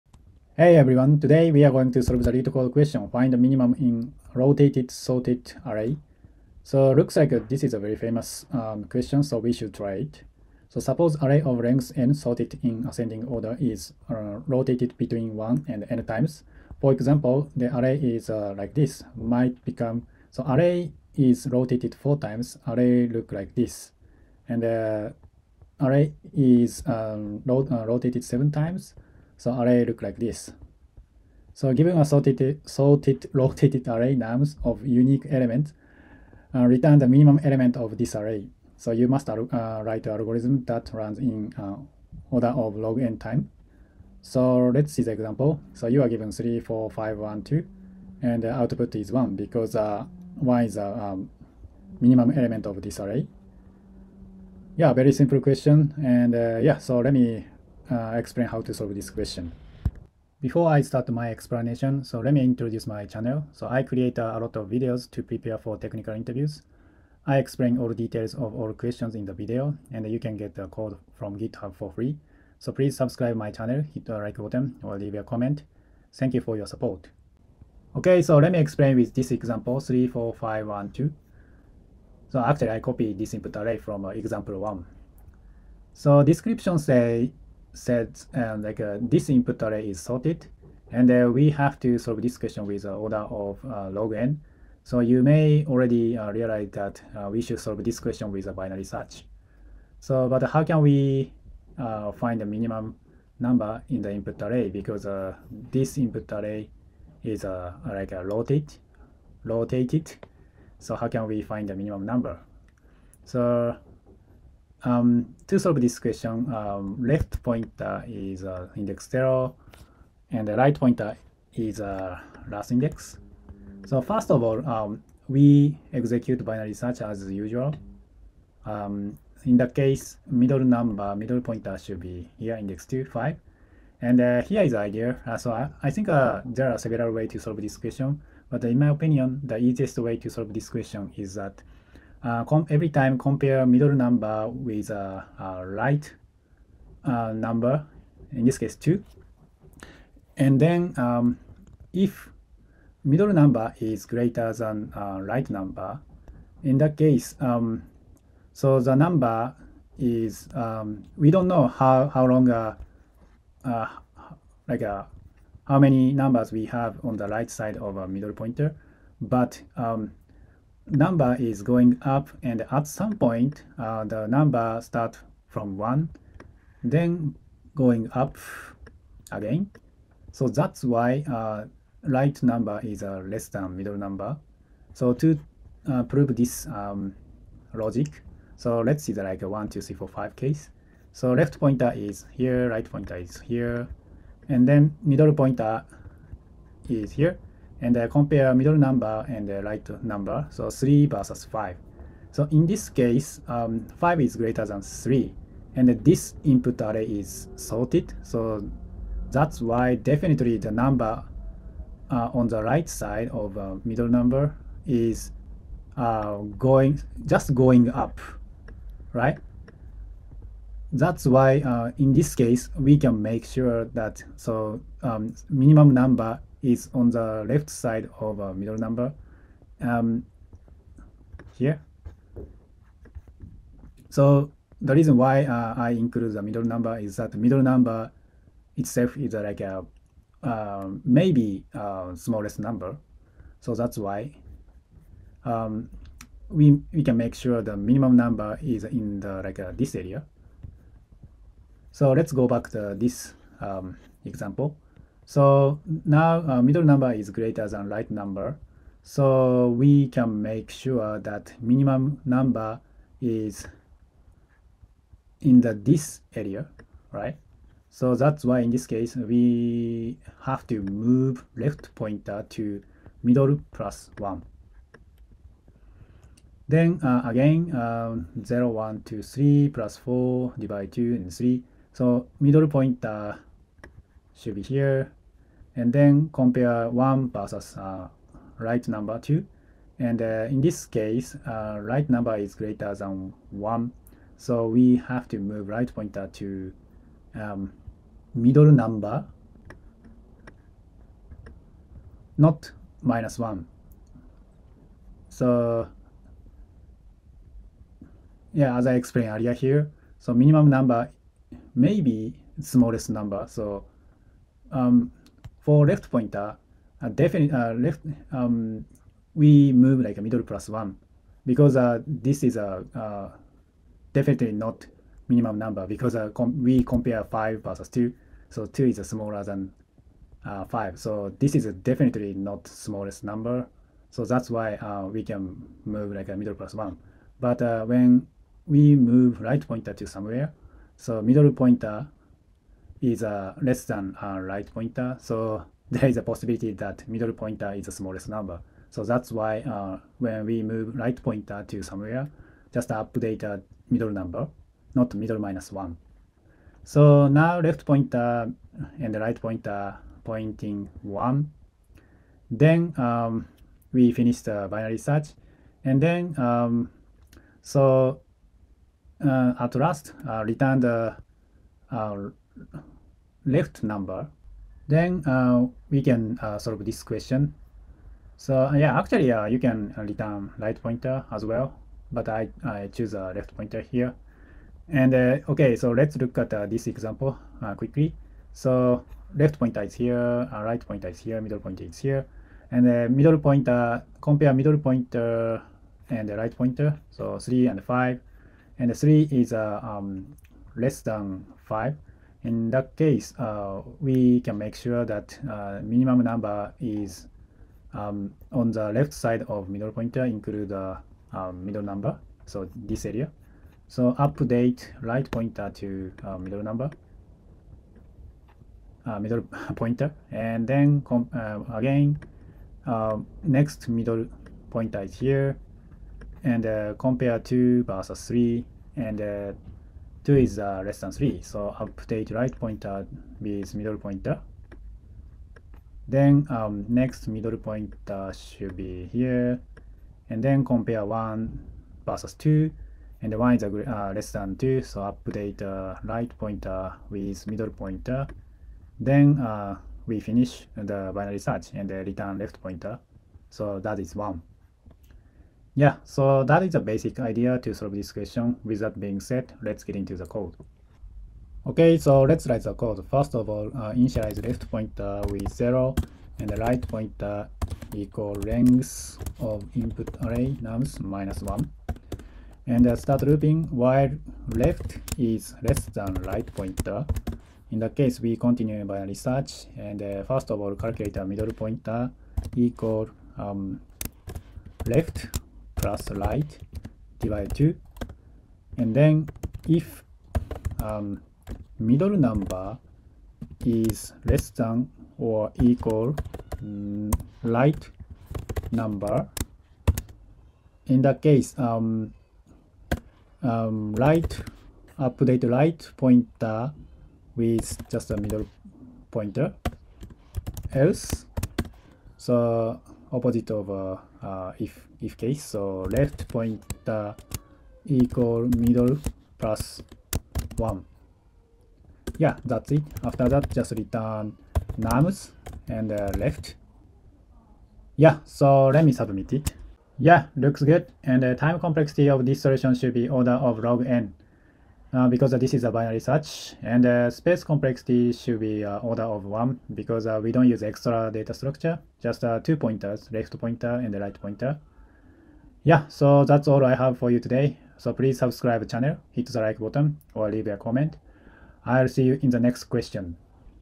Find Minimum in Rotated Sorted Array - LeetCode #153 | Python, JavaScript, Java, C++
Let's solve LeetCode #153 Find Minimum in Rotated Sorted Array!

■ Timeline
0:00 Read the question of Find Minimum in Rotated Sorted Array
1:17 Explain a basic idea to solve Find Minimum in Rotated Sorted Array
7:41 Coding
8:49 Time Complexity & Space Complexity